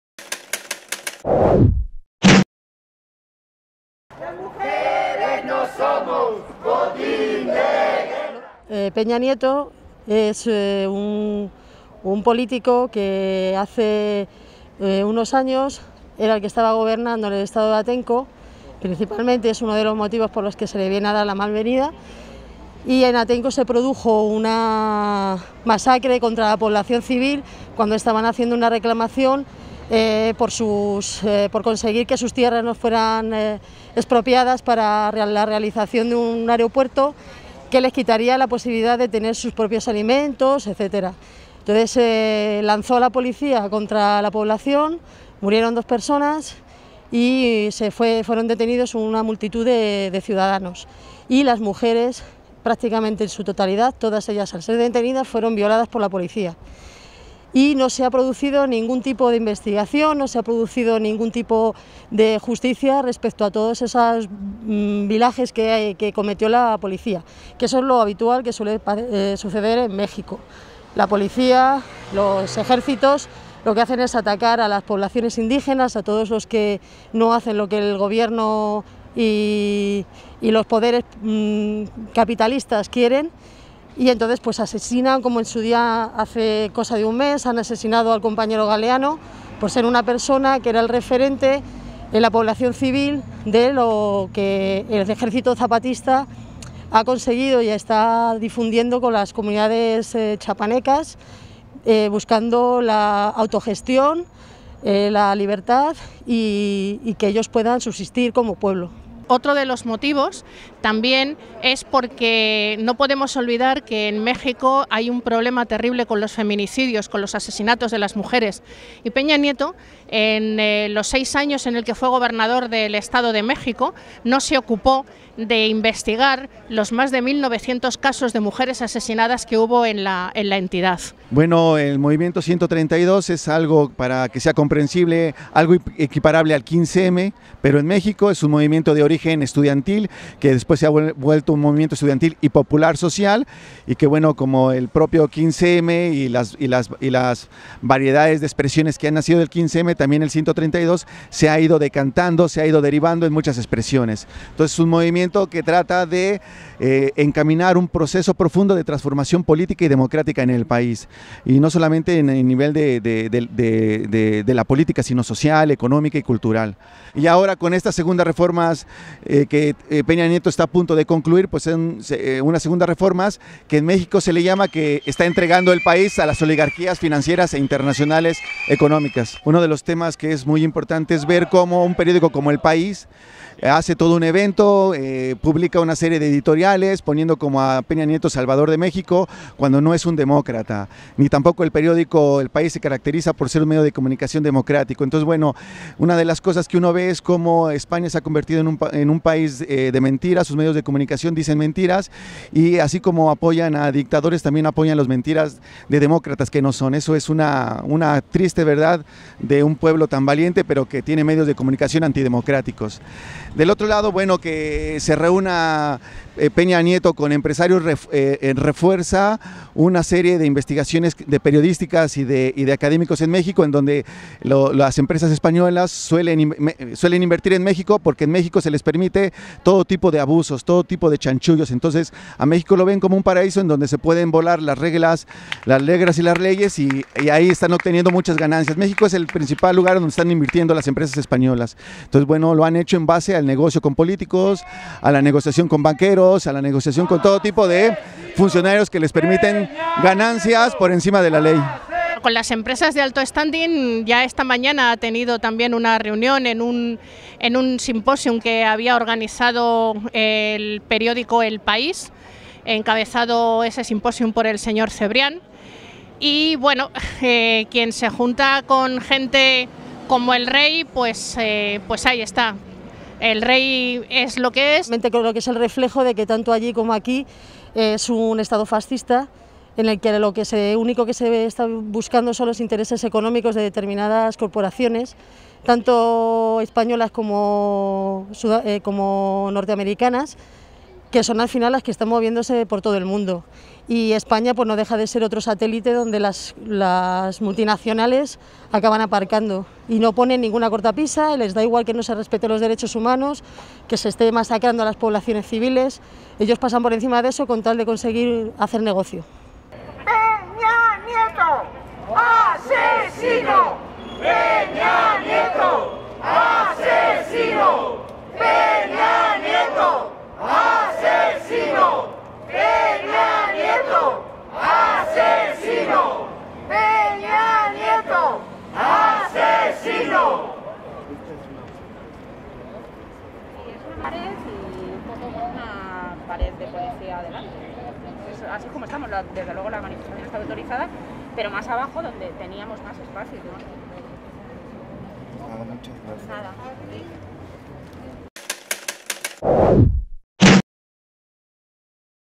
Testimonios en la Malvenida a Peña Nieto
Peña Nieto llega a Madrid para reunirse con las oligarquías españolas para sus negocios, un grupo de simpatizantes con la causa de Chiapas le reciben con una gran malvenida.
La policía impidió que el desarrollo de la protesta se realizara en el lugar solicitado y aprobado por la delegación del gobierno.